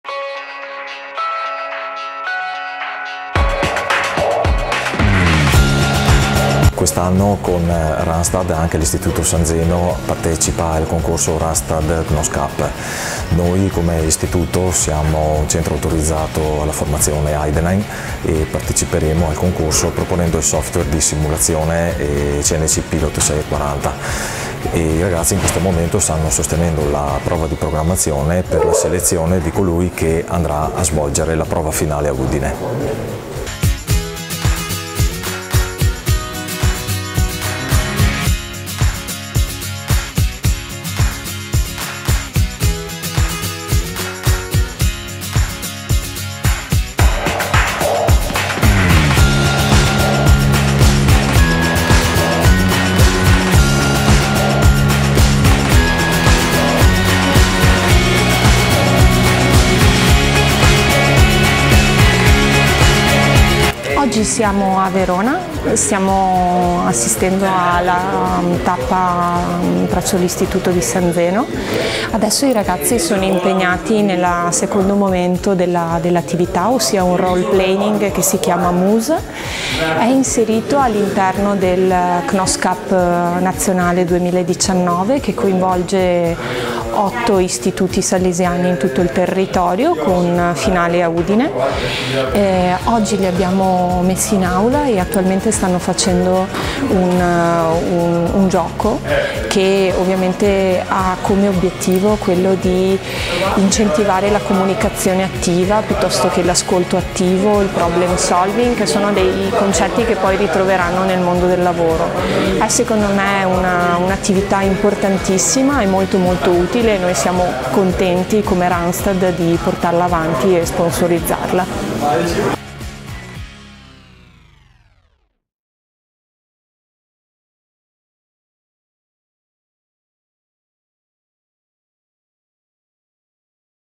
Randstad CNOS-FAP Cup 2019 - tappa all'Istituto Salesiano San Zeno
L’iniziativa promossa da Randstad in partnership con CNOS-FAP e  i Centri di Formazione Salesiani presenti in Italia, che hanno tra le loro offerte formative quella degli operatori meccanici con macchine tradizionali e a controllo numerico.

Un tour in otto tappe con partenza da Bra martedì 29 gennaio alla ricerca dei migliori aspiranti tornitori, professionalità molto richieste dal mercato, ma di difficile reperibilità con le adeguate competenze.

Nel corso delle tappe, gli studenti della Formazione Professionale Salesiana si sfideranno in esercizi di programmazione CNC su tornio.

Nel video l'esperienza della tappa all'Istituto Salesiano San Zeno di Verona.